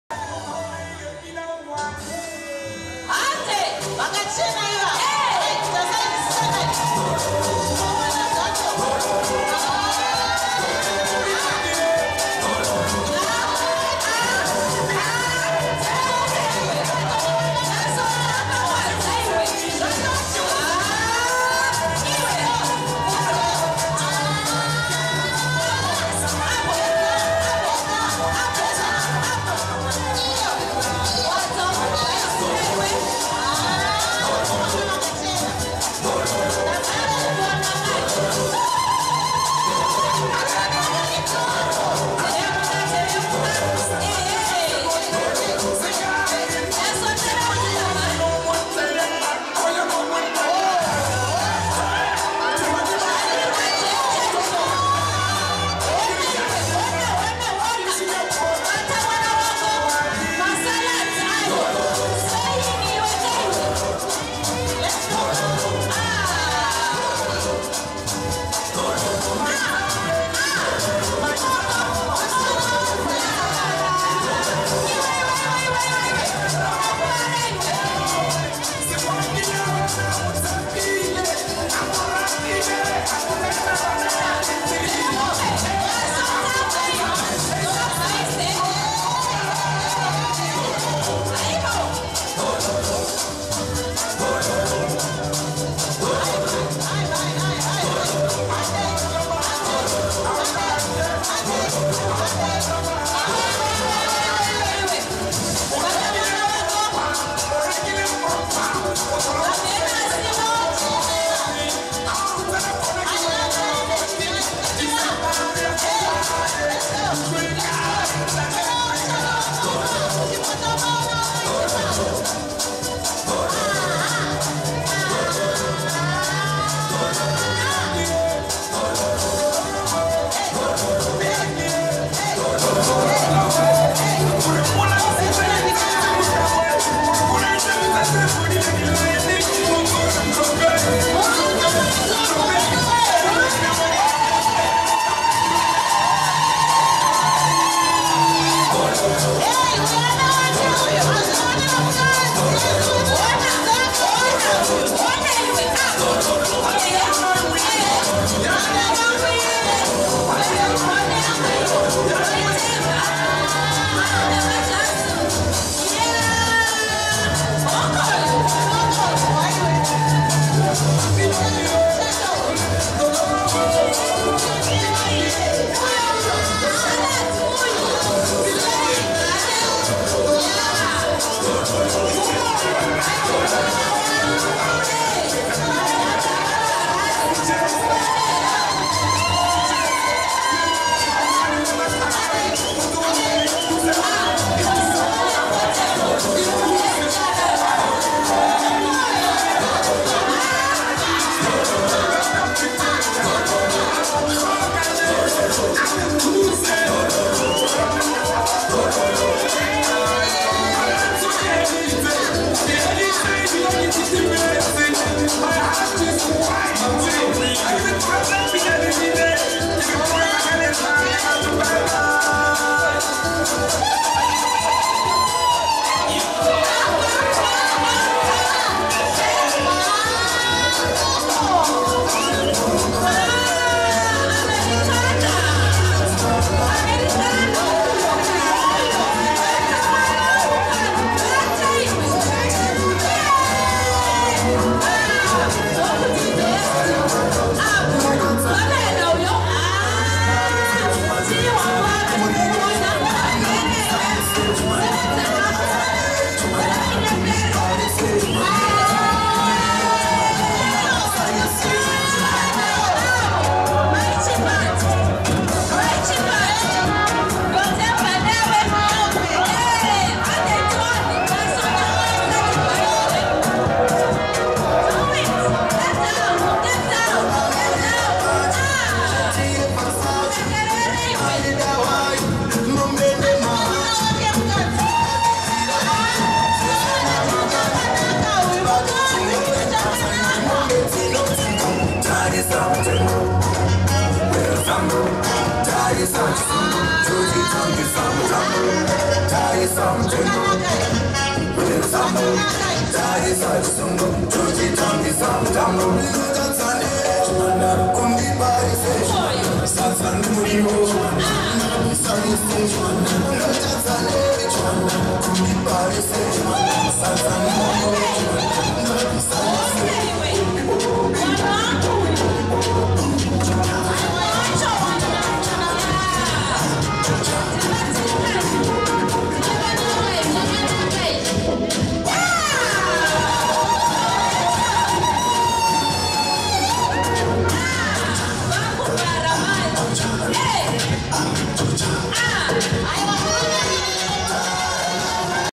Pearl & Farai Wedding Dance
What a dance by this bridal team.Hope you enjoy watching it.